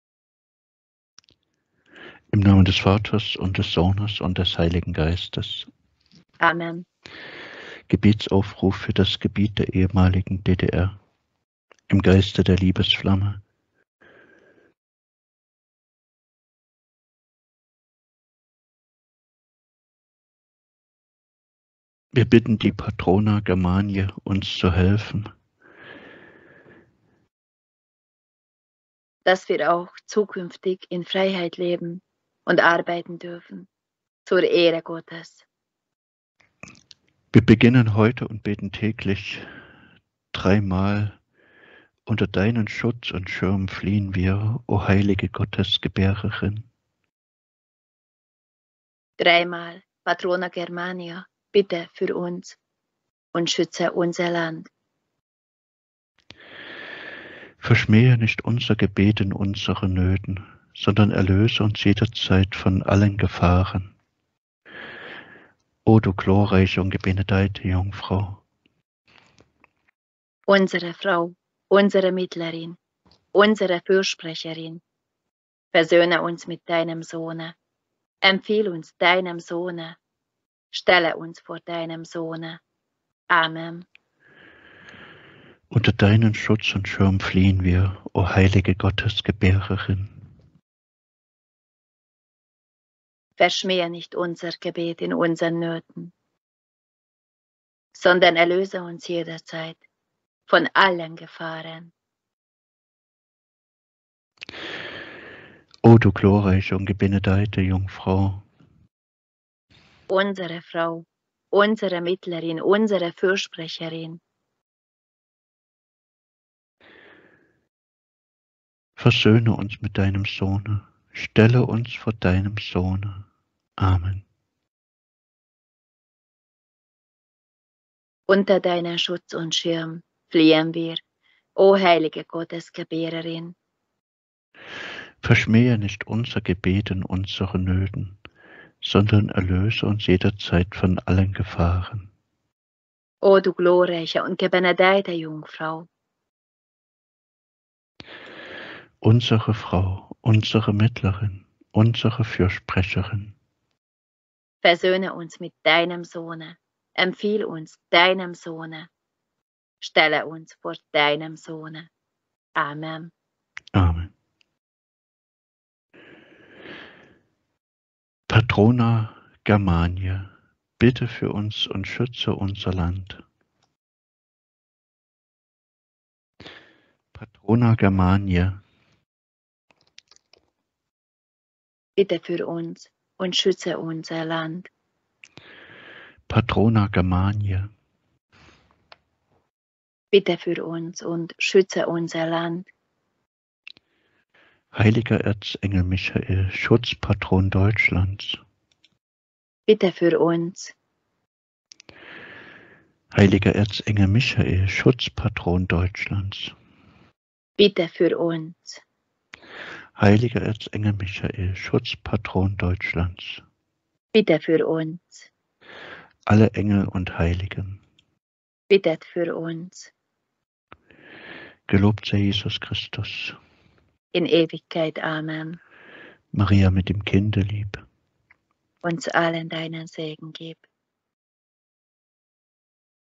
GEBETSAUFRUF für das Gebiet der ehemaligen DDR
Wir bitten die Patrona Germaniae uns zu helfen,
dass wir auch zukünftig in Freiheit leben
und arbeiten dürfen zur Ehre Gottes.

Wir beginnen heute und beten täglich:

3 mal „Unter Deinen Schutz und Schirm ….

3 mal. „Patrona Germaniae, bitte für uns und schütze unser Land“

Hl. Erzengel Michael, Schutzpatron Deutschlands, bitte für uns

Alle Engel und Heiligen, bittet für uns.

Jeder, der möchte, kann sich diesem Gebetssturm anschließen und heute beginnen.

Vergelt´s Gott

Unter deinen Schutz und Schirm fliehen wir,
o heilige Gottesgebärerin.
Verschmähe nicht unser Gebet in unsern Nöten,
sondern erlöse uns jederzeit von allen Gefahren,
o du glorreiche und gebenedeite Jungfrau.
(Unsere Frau, unsere Mittlerin, unsere Fürsprecherin.
Versöhne uns mit deinem Sohne,
empfiehl uns deinem Sohne,
stelle uns vor deinem Sohne.)
Amen.